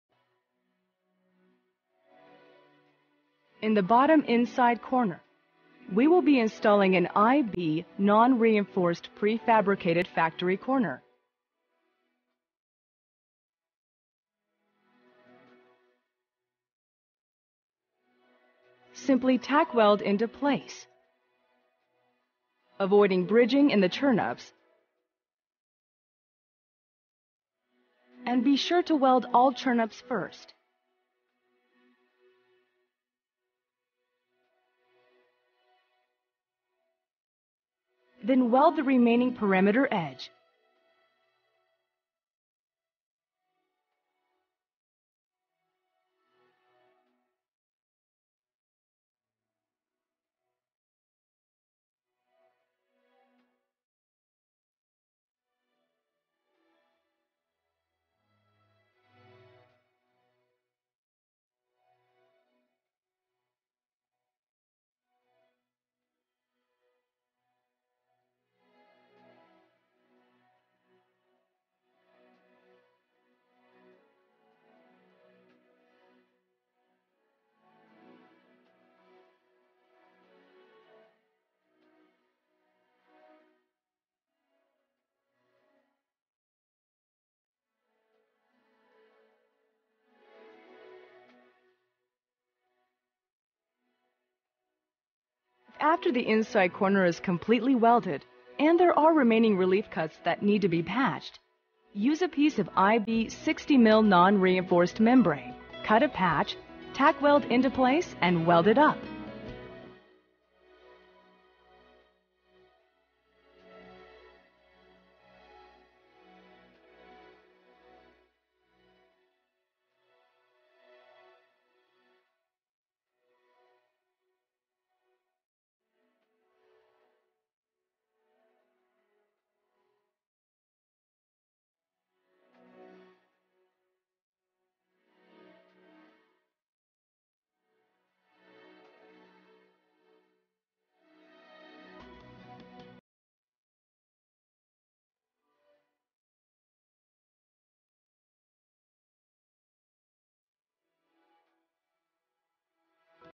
Flat roof Installation - Part 10 - Inside Corner
Inside corner installation detail using a prefabbed corner flashing, fusion welded into place with hot air gun.